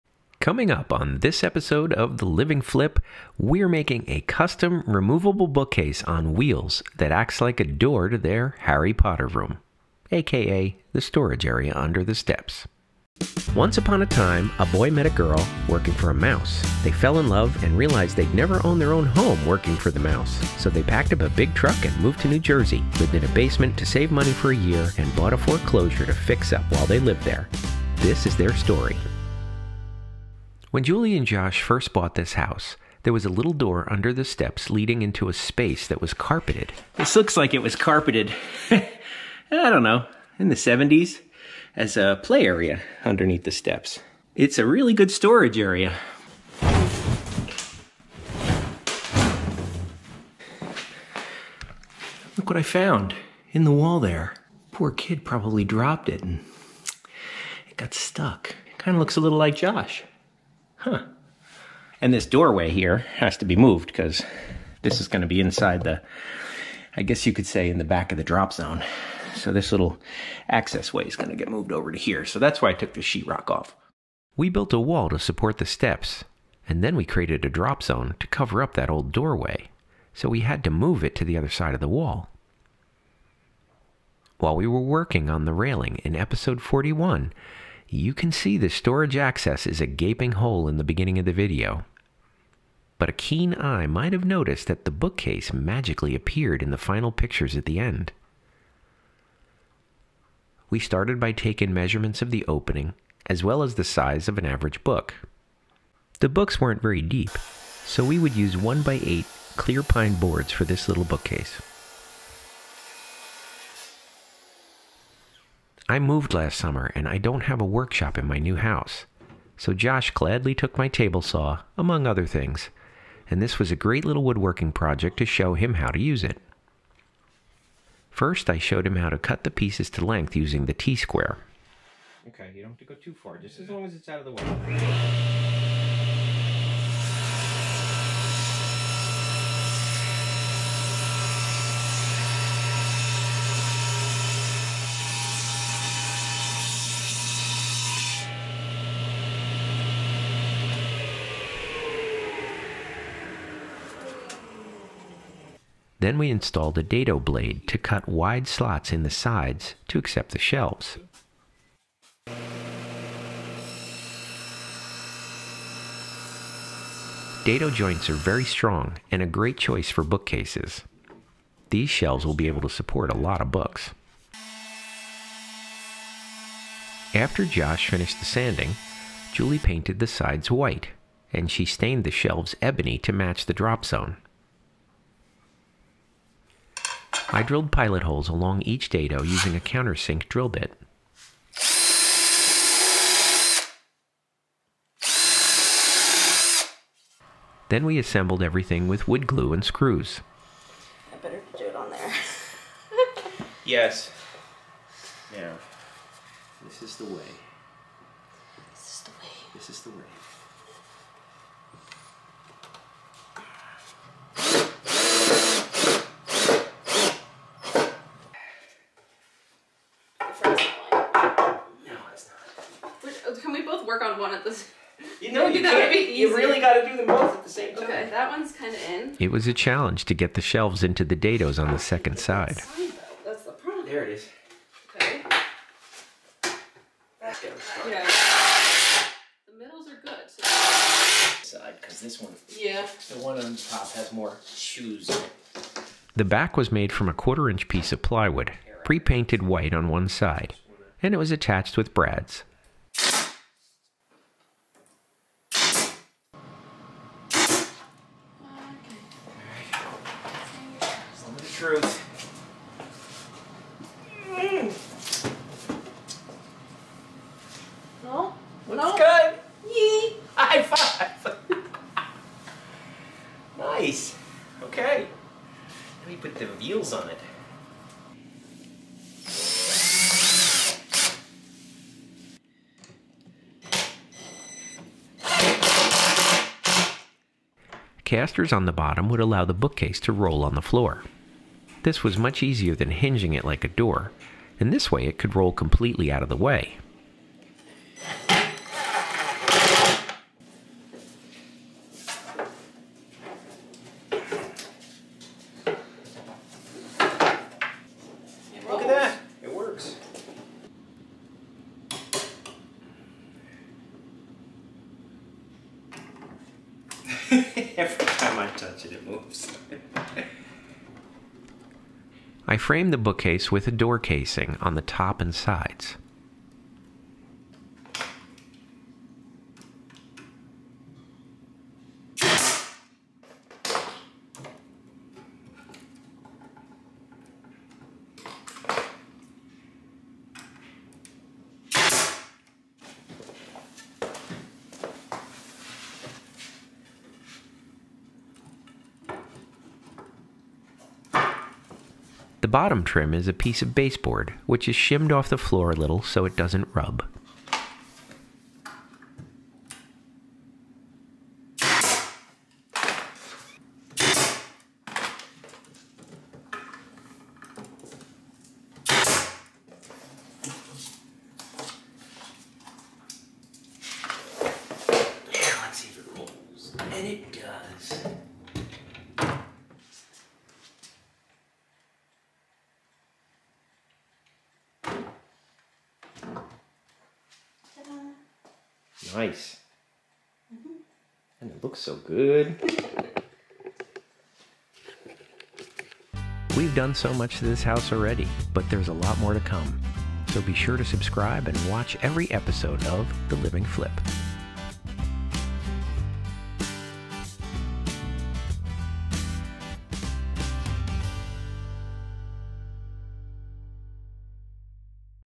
WE BUILT A SECRET BOOKCASE // TLF 44
In this video, Chris helps Josh build his first woodworking project: a hidden bookcase door on wheels that conceals the storage area under their steps.

This is episode 44 of The Living Flip, a series where my daughter Julie and her husband Josh bought a foreclosure to fix up while they live there.